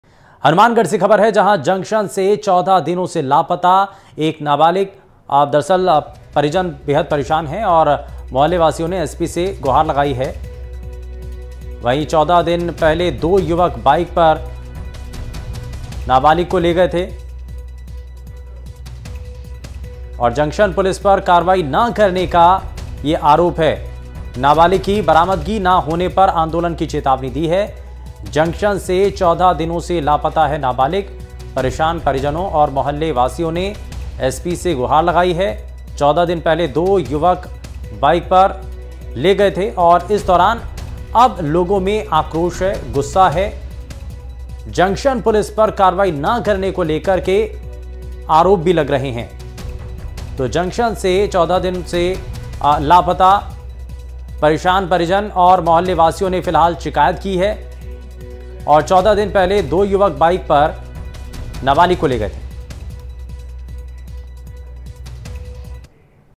Hanumangarh: जंक्शन से 14 दिनों से लापता एक नाबालिग, परिवार ने Police पर लगाई गंभीर आरोप। Rajasthan
Hanumangarh Kidnapping: राजस्थान (Rajasthan) के हनुमानगढ़ में जंक्शन से 14 दिनों से एक नाबालिग लापता है. लापता होने के बाद परेशान परिजनों और मोहल्ले वासियों ने एसपी (SP) से गुहार लगाई. बता दें कि 14 दिन पहले दो युवक नाबालिगा को बाइक पर ले गए थे. परिवार के लोगों ने पुलिस (Police) पर कार्रवाई न करने का आरोप लगाया है. वहीं परिजनों ने यह भी कहा कि जंक्शन (Junction) के लोग उन्हें धमकी दे रहें हैं.